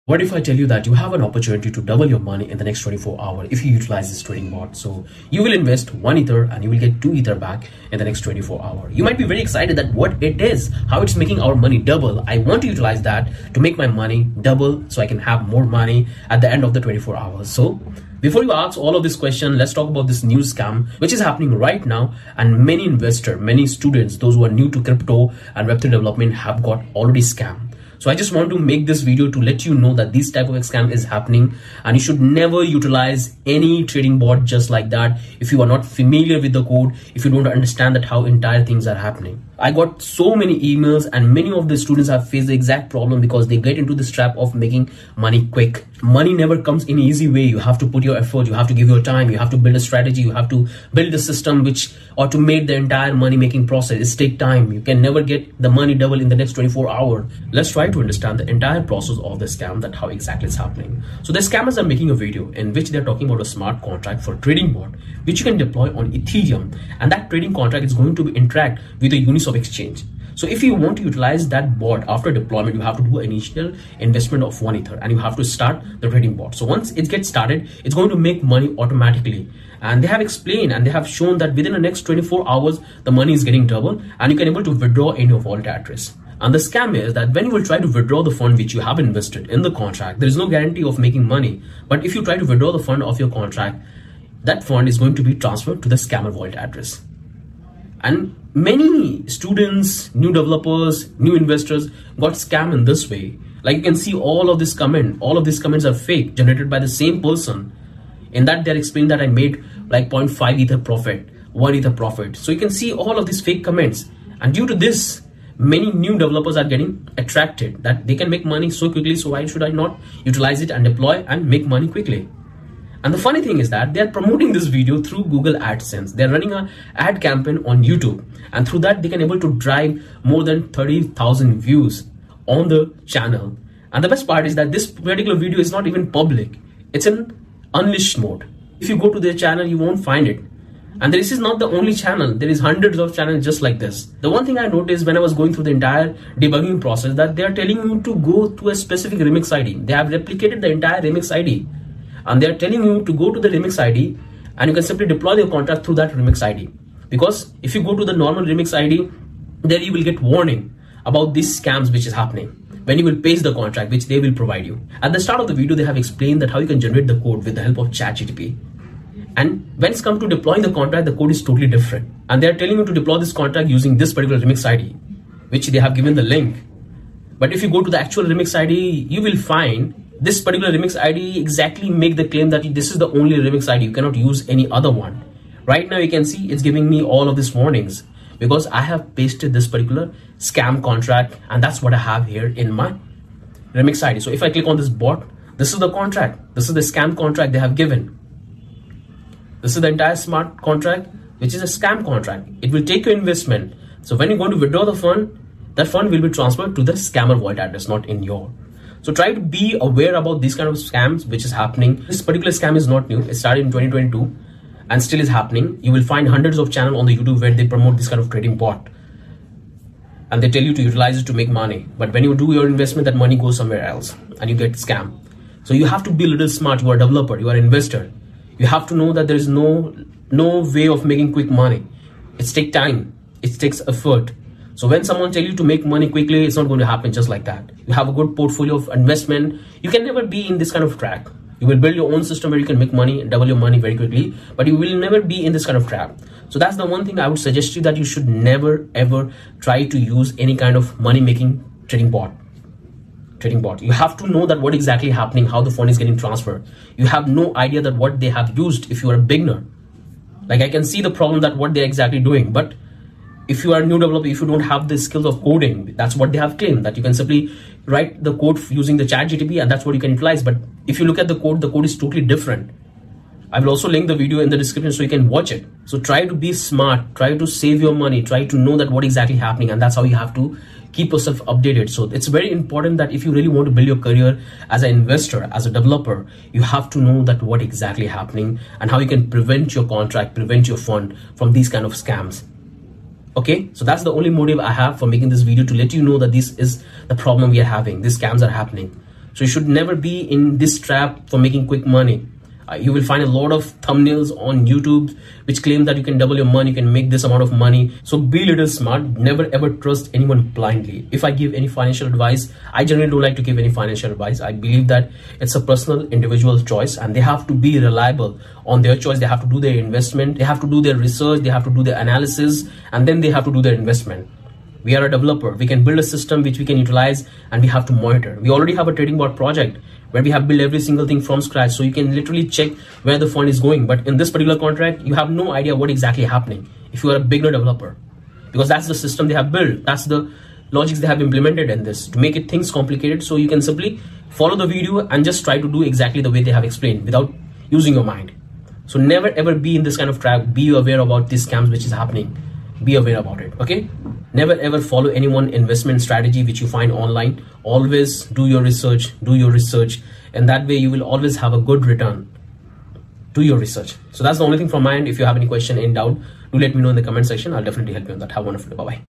How to Double Your ETH with a Trading Bot | Beware of Crypto Trading Bot Scams
In this video, we dive into the world of cryptocurrency trading bots, specifically those promising to double your ETH. While these bots might sound like an easy way to make quick profits, they often come with significant risks and potential scams. We'll explore how these trading bots operate, the red flags to watch out for, and real-life examples of scams. Learn how to protect yourself and make informed decisions in the volatile world of crypto trading. Don't get caught in the trap—stay safe and informed!

Workout Video: